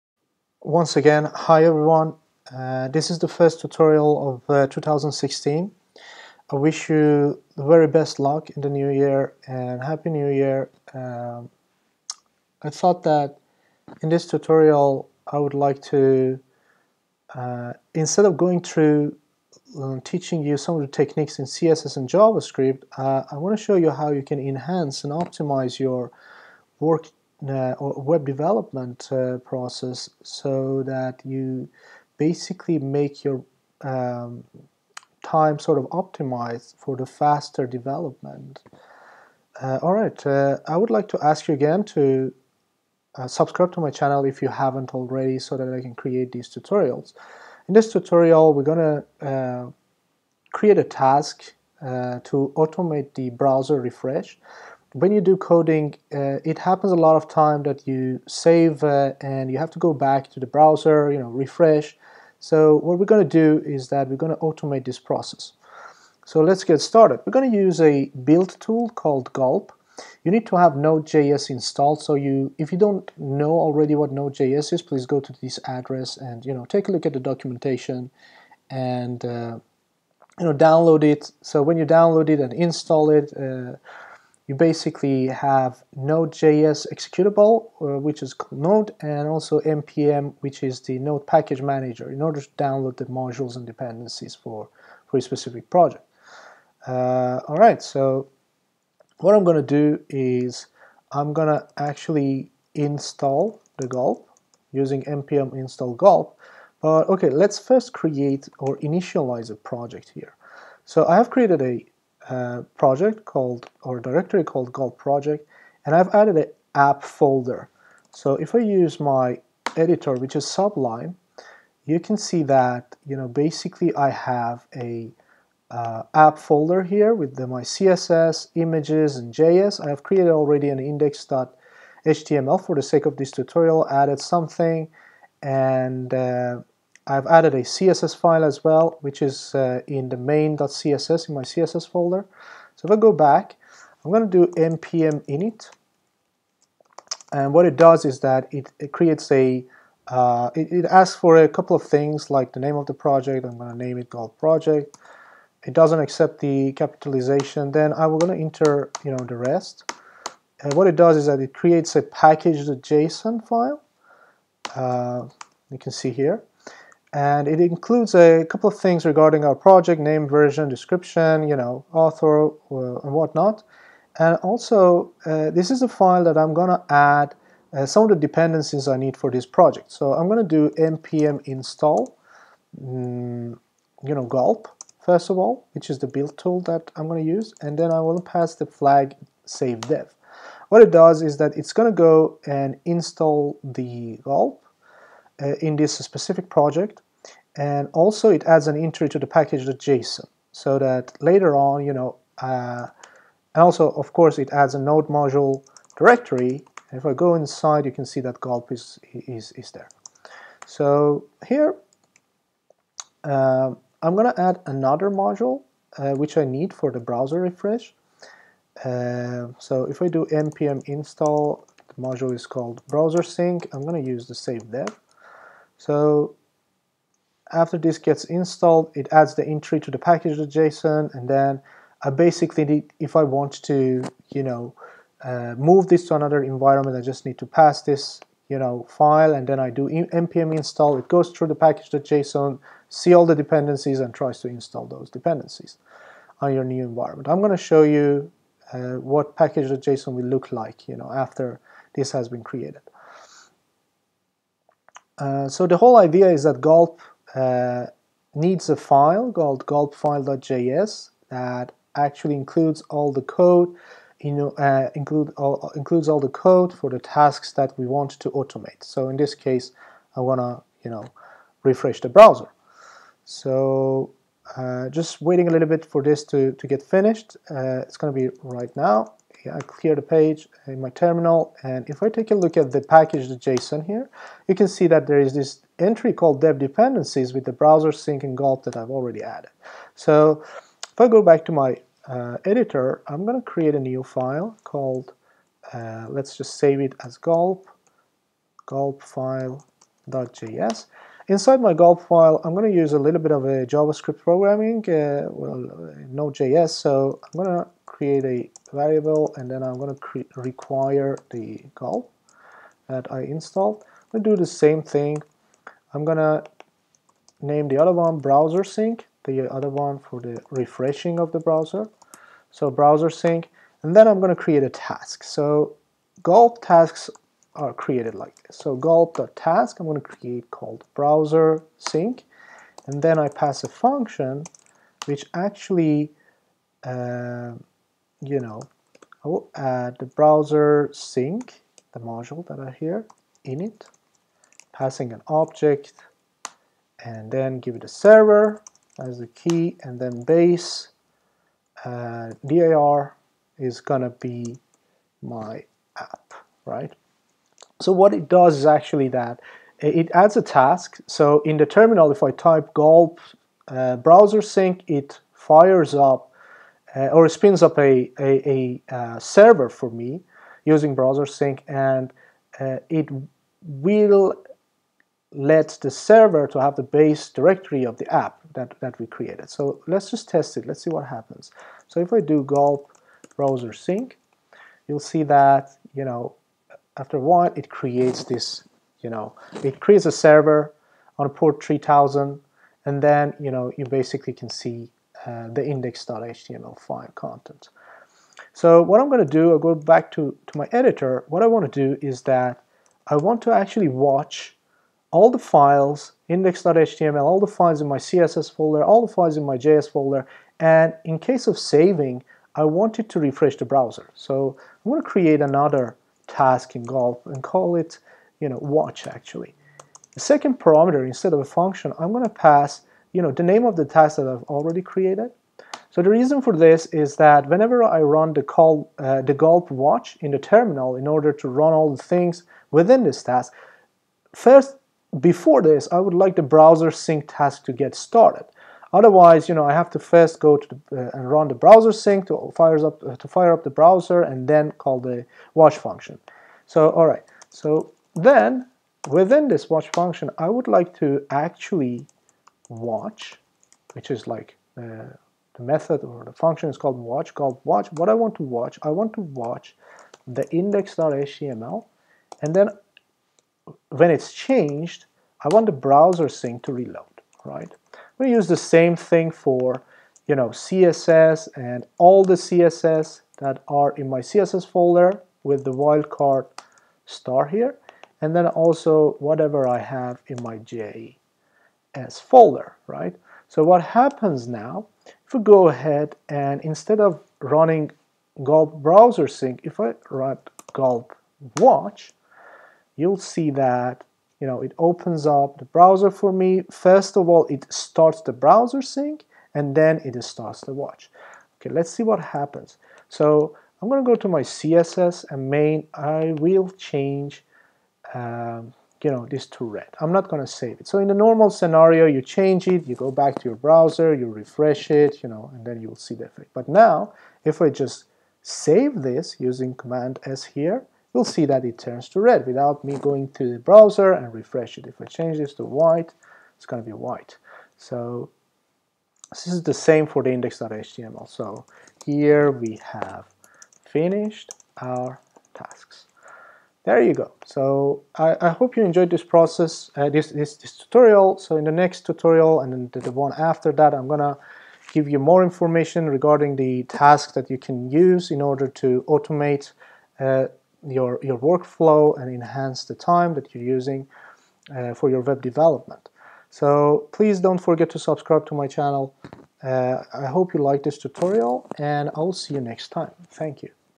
Optimize your web development workflow Part 1: Browser auto refresh on saving the code
In this tutorial I will show you how you can enhance your web development process by automate some of the tasks that you do during your coding.

We will use gulp and browser-sync to automate the refreshing of the browser. When coding, a lot of times we save the files and go back to browser, refresh it to see the changes. Using this workflow you remove this barrier.